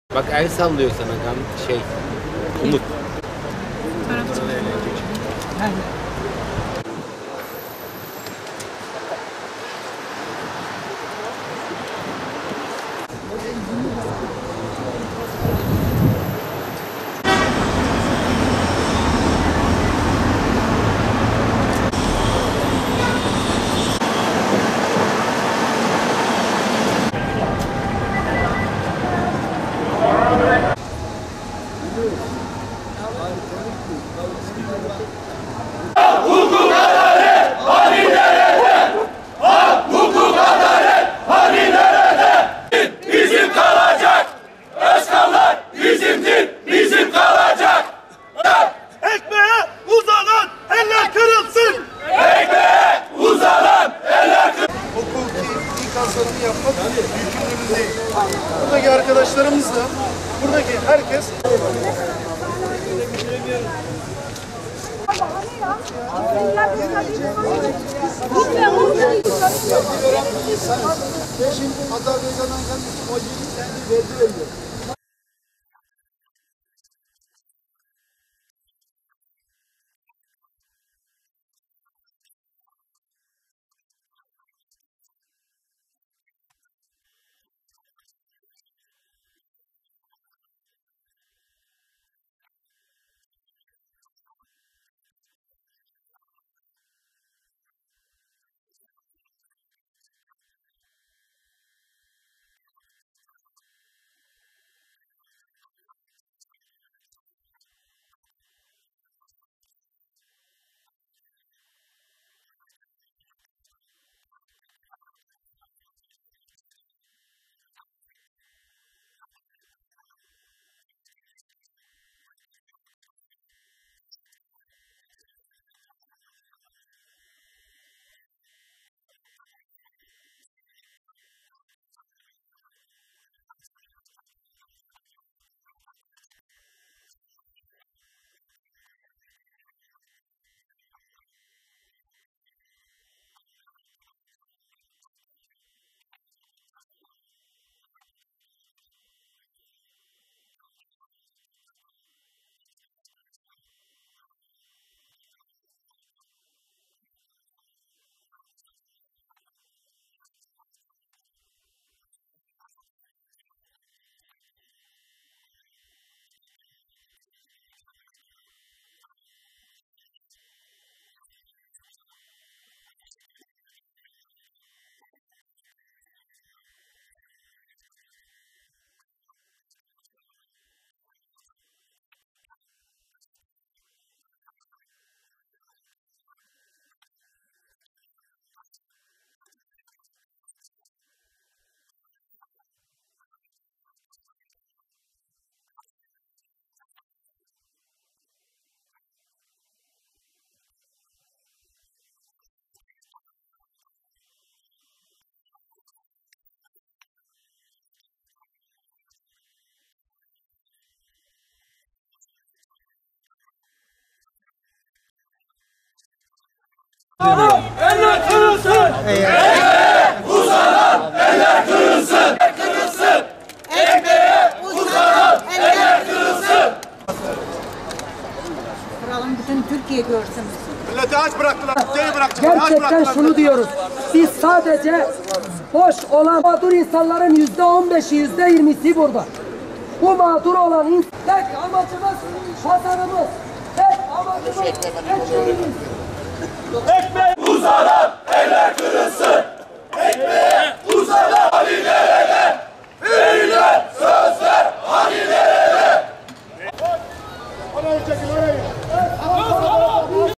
Belediye önünde oturma eylemi
İzmir'in Bayraklı ilçesinde yıllarca hizmet verdikten sonra şikayet üzerine Danıştay kararıyla kapatılan Özkanlar Pazar Yeri esnafına gösterilen yeni bölge için ruhsat verilmediği iddia edildi. Esnaf, Bayraklı Belediyesi önünde toplanarak oturma eylemi yaptı ve mağdur olduğunu ifade etti.